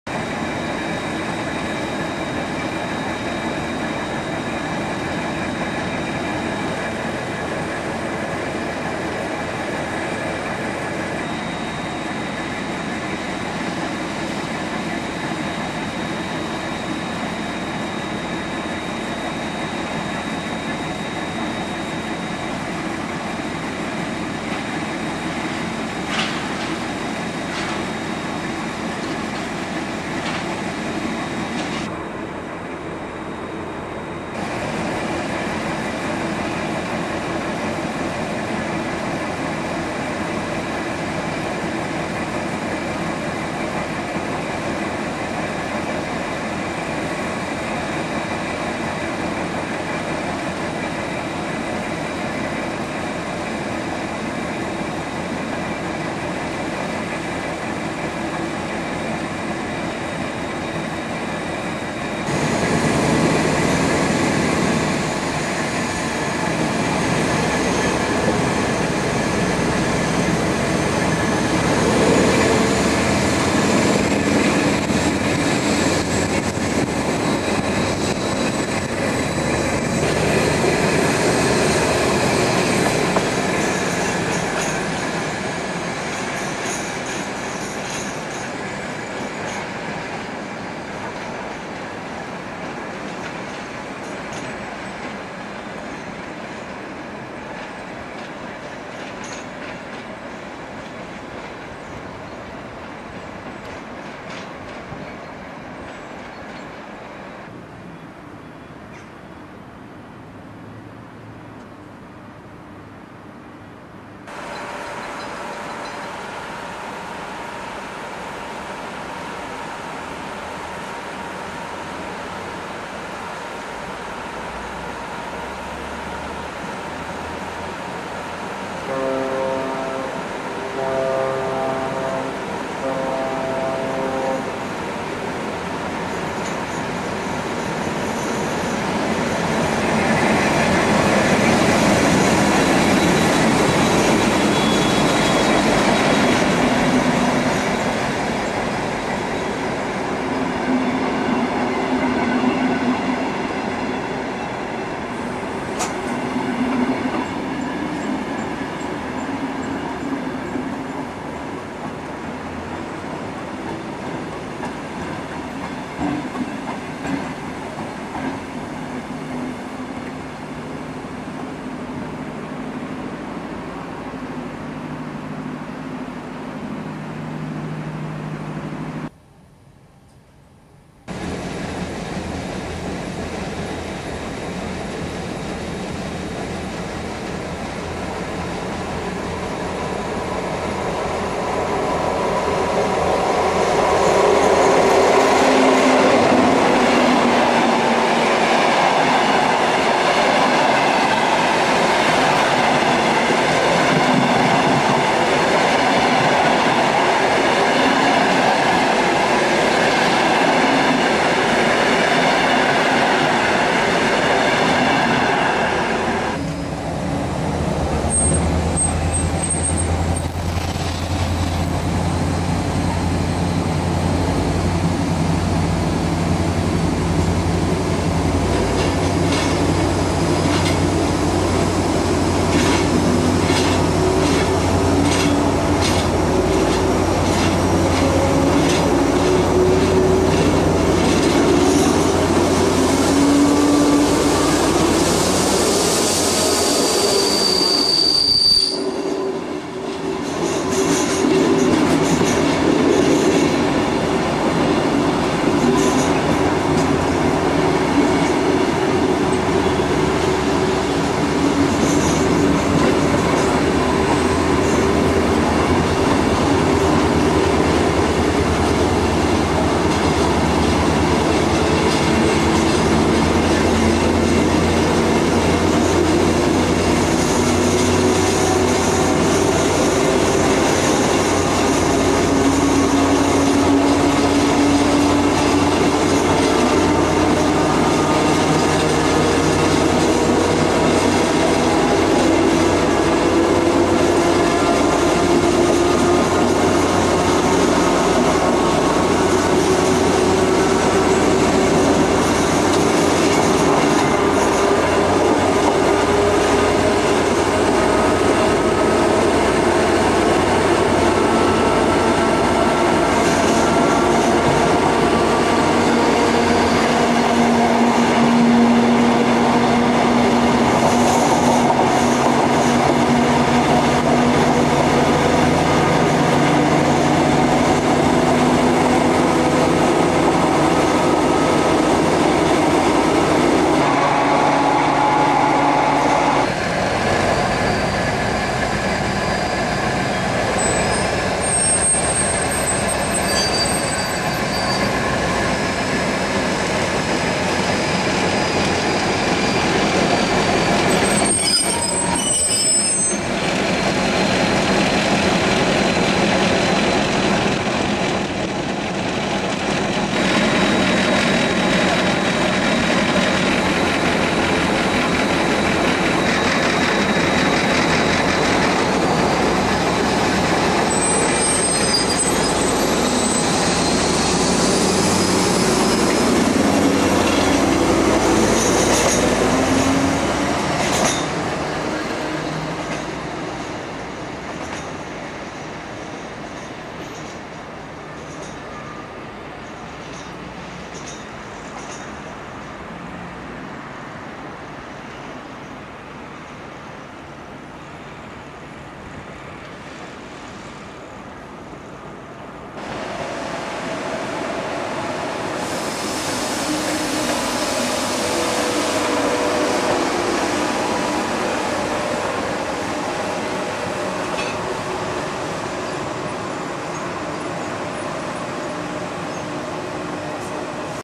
VR Dr13 2311 11-10-99
We start with an overview of Turku shed, with plenty of power on show and then Dr13 2311 propels empty stock.

Next, we board train P807, the 09.45 to Pieksamaki for a run to Tampere with 2311. En route we depart the amusingly named (to me) station of Humppila, where the driver has to recover from a big wheelslip.

Finally at Tampere the loco is removed from the train.

In my mind's eye the Dr13s are very odd looking creatures. The body styling, the bogies, the twin engine format - just bizarre! But while the exhaust note doesn't exactly set the pulse racing, they seem solid machines, just like the other VR locos.

All bar 2 have been scrapped - apparently they were withdrawn from service the following year.